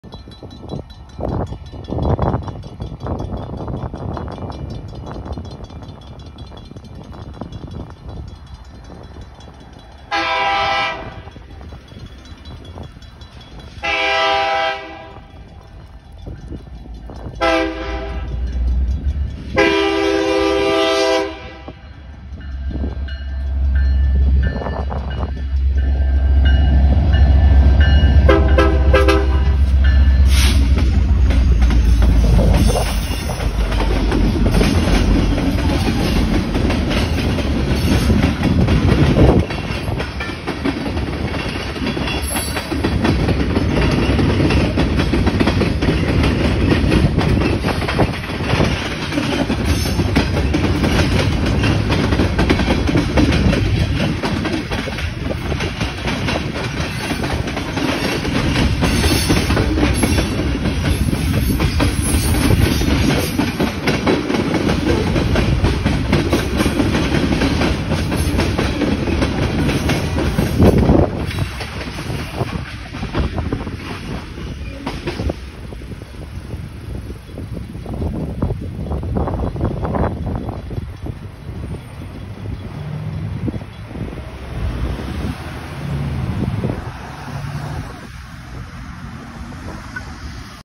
BNSF 102 leads the 748 Job back towards Watson Yard after passing Sepulveda Blvd
Taken just after 3:30pm with 102 leading and 145 trailing and I got a video of them as they were heading towards Watson Yard from the Alcoa Yard and El Segundo Yard and the train is carrying one slab car and some oil tanker cars that are all heading towards Watson Yard and thanks to the crew of both locomotives 102 for waving and horning at me and the crew of 145 for waving at me and enjoy the video everybody